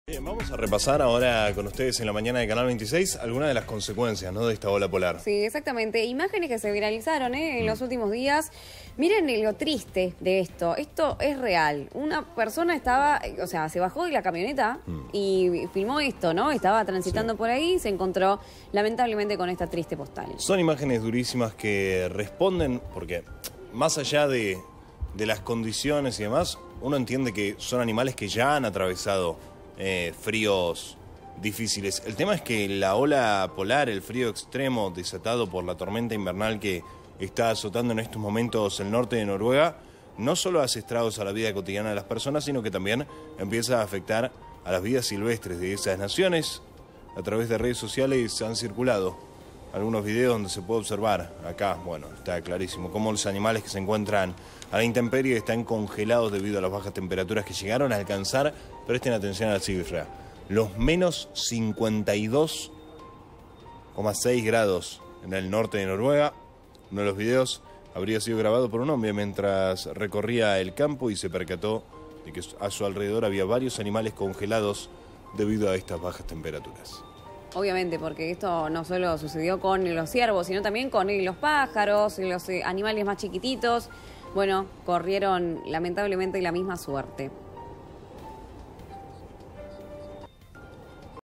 NORUEGA | Las imágenes de los animales que quedaron congelados por el frío extremo
👉  El país atraviesa momentos de temperaturas bajo cero nunca antes registradas